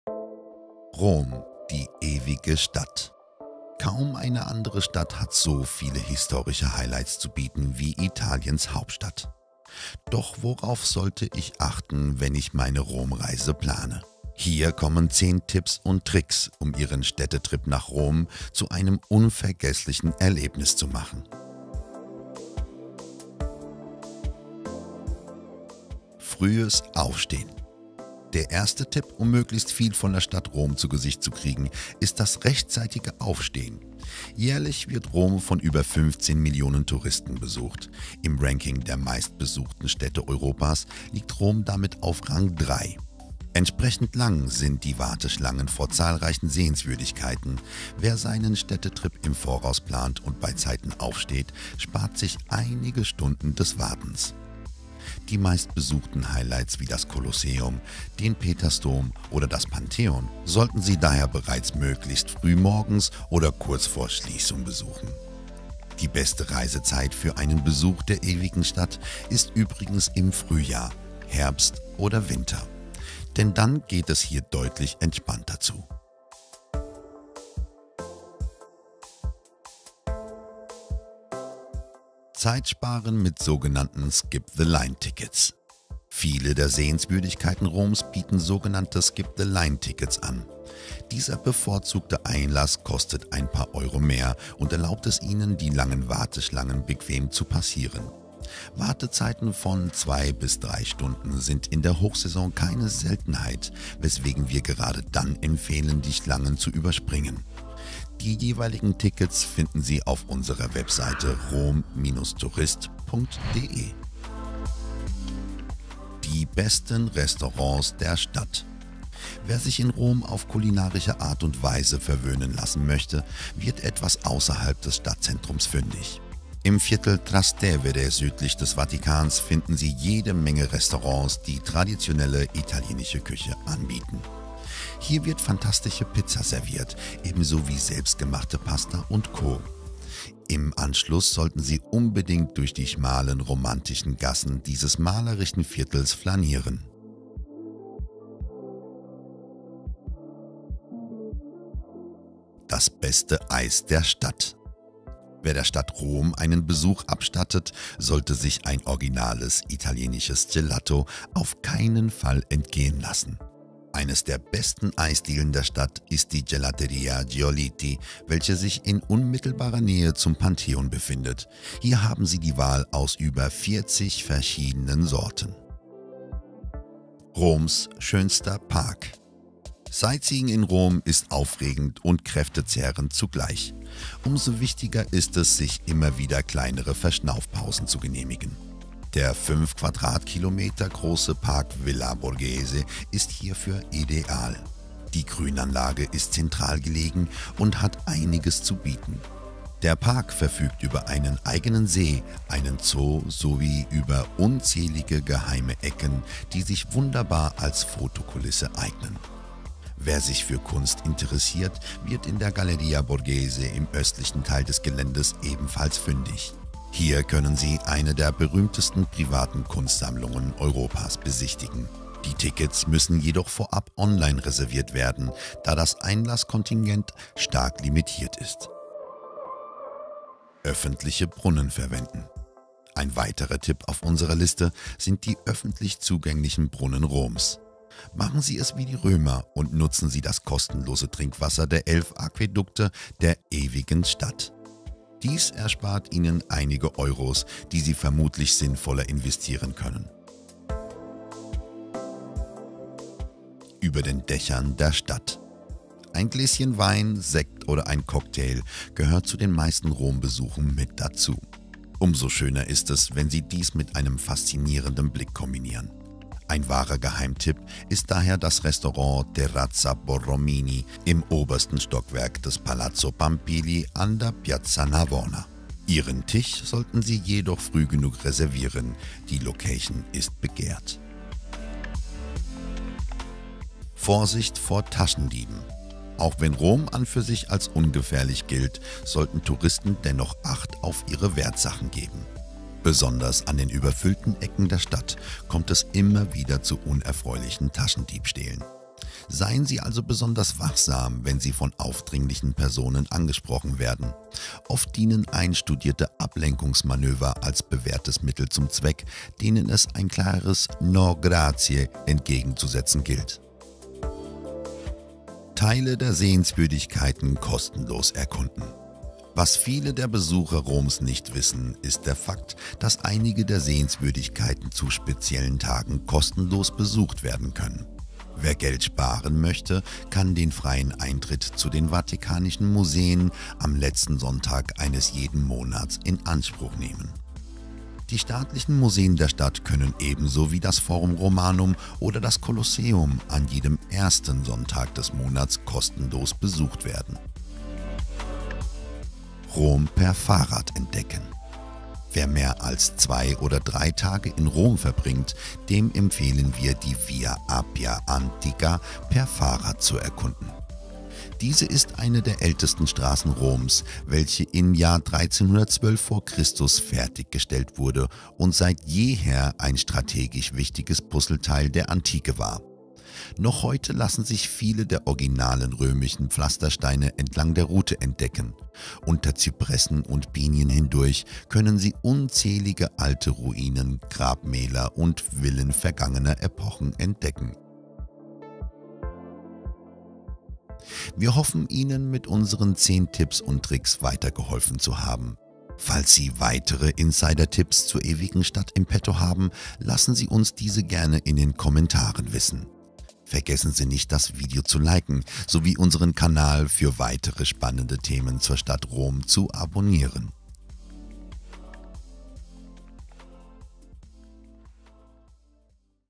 10 Tipps und Tricks für Rom 🌏 - Das solltest du für deine Städtereise wissen!
▬ Inhalt: ▬▬▬▬▬▬▬▬▬▬▬▬▬▬▬▬▬▬▬▬▬▬▬▬

0:00 Einleitung
0:24 Früh aufstehen
1:21 Warteschlangen überspringen
1:55 Die besten Restaurants
2:30 Die beste Eisdiele
2:56 Der schönste Park
3:48 Kostenloses Trinkwasser
4:12 Restaurant Tipp Piazza Navona
4:42 Vorsicht vor Taschendieben
5:13 Geld sparen
5:47 Via Appia Antica

▬ Die 10 besten Reisetipps für Rom: ▬▬▬▬▬▬▬▬▬▬▬▬

In diesem Video präsentieren wir Ihnen 10 tolle Tipps & Tricks für Ihre Rom Reise. Mit dabei sind einige Geheimtipps. Sie erfahren beispielswiese, von welchem Restaurant aus Sie einen exklusiven Blick auf Rom haben und dabei ein brillantes Essen serviert bekommen.

Zudem erfahren Sie, wie Sie die langen Warteschlangen an einigen der meist besuchten Sehenswürdigkeiten der Stadt umgehen. Gerade in der Hochsaison sind Wartezeiten von 2 Stunden keine Seltenheit.

Wenn das Schlemmen von leckerer Eiscreme für Sie dazugehört, dann werden Sie mit unserem Tipp zur besten Eisdiele Roms voll auf Ihre Kosten kommen. Denn hier kommen nicht nur zufällig vorbeilaufende Touristen her, sondern auch ranghohe Politiker aus dem benachbarten Parlament.

Wer sich nach einer kurzen erholsamen Auszeit vom Touristenandrang Roms sehnt, wird mit diesem Tipp zufrieden sein. Wir zeigen Ihnen den vermutlich schönsten Park der Ewigen Stadt, welcher zudem eines der bekanntesten privaten Kunstgalerien ganz Europas beherbergt. Der Zoo gleich um die Ecke macht den Park besonders für Familien mit Kindern attraktiv.

Eines der praktischsten Tipps für Rom Reisende, ist die Nutzung der öffentlichen Brunnen der Stadt. Bei sommerlichen Temperaturen kann eine Städtereise auf Dauer schnell anstrengend werden. Daher ist es umso wichtiger, genügend Wasser zu trinken.

Zwar ist Rom sicher, vor Taschendieben sollte man sich dennoch in Acht nehmen. Mit unseren Tipps bleiben Sie achtsam, und bei Bedarf auf der Lauer.

Wer möchte nicht etwas Geld sparen? Zu bestimmten Zeiten im Monat, sind einige der Top Sehenswürdigkeiten in Rom kostenlos betretbar. Wann genau Sie in den Genuss dieses Angebots kommen, erfahren Sie in unserem Video